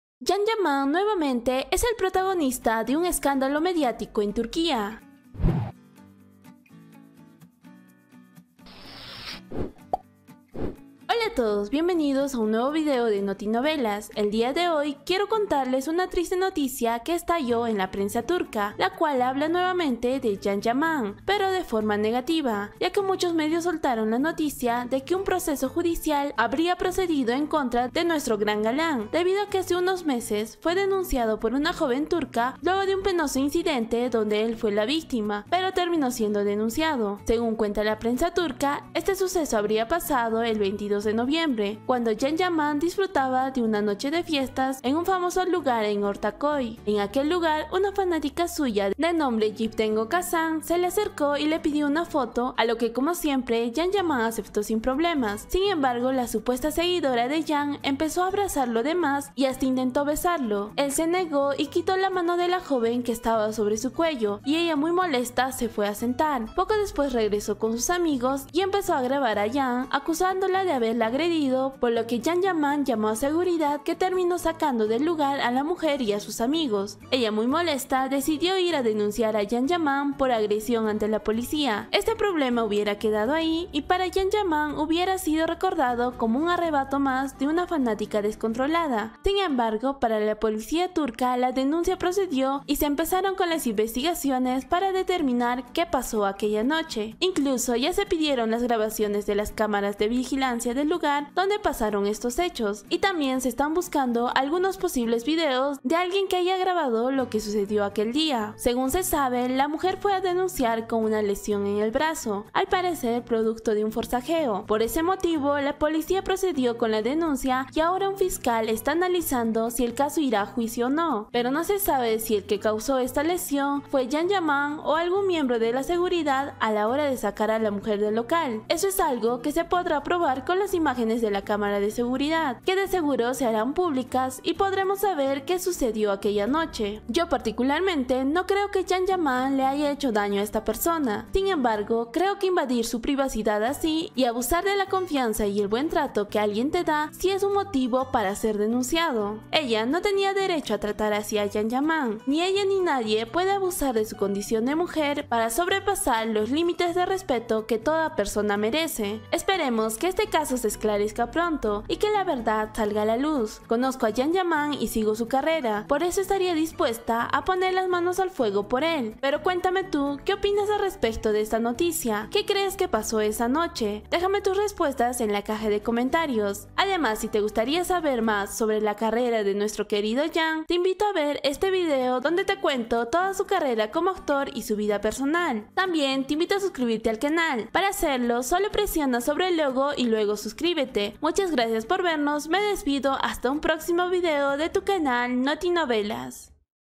Can Yaman pasa por TERRIBLE MOMENTO por culpa de una MALA FAN
Hola a todos, en este video les cuento lo que paso con Can Yaman y por que todos hablan de el nuevamente.